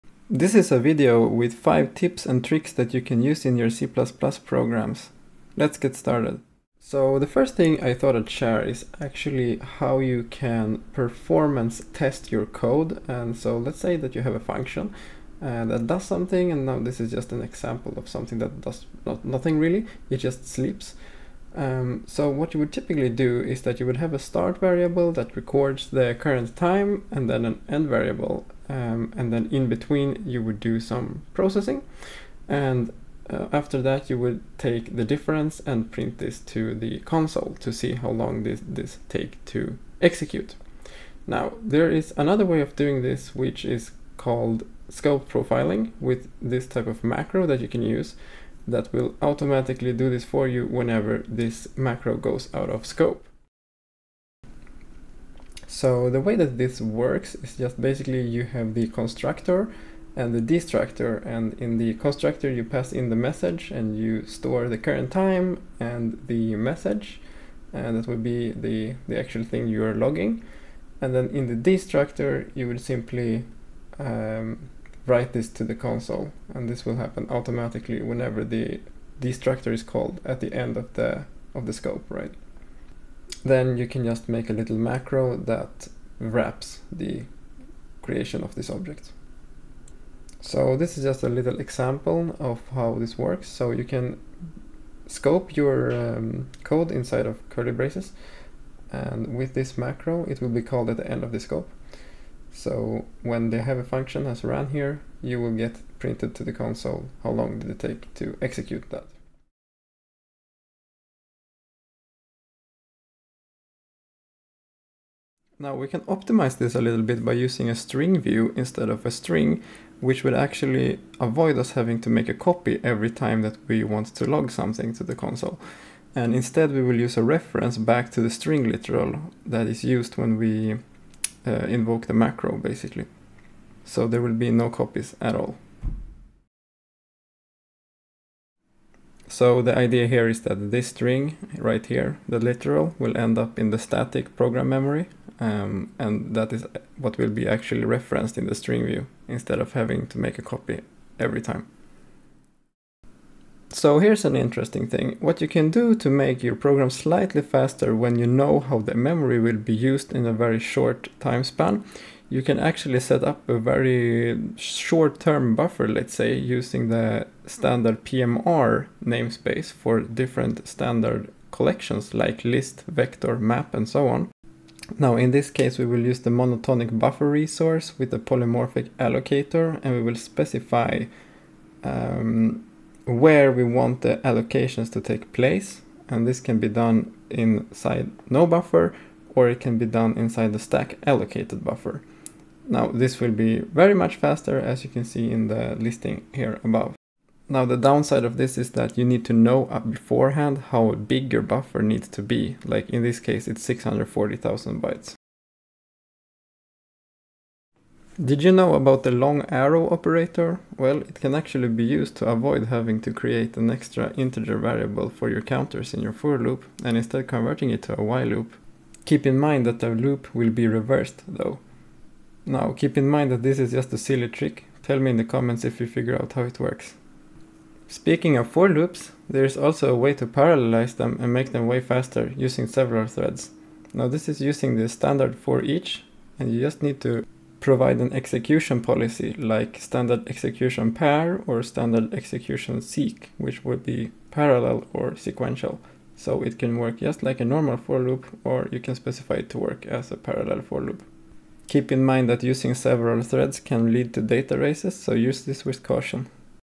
5 C++ Need to Know Tips/Tricks!
1. How you can profile your C++ code easily.
2. How to use optimize your code using string_view.
3. Fast polymorphic memory resource allocations for std:: containers.
4. The silly long arrow operator.
5. Parallelized for loops.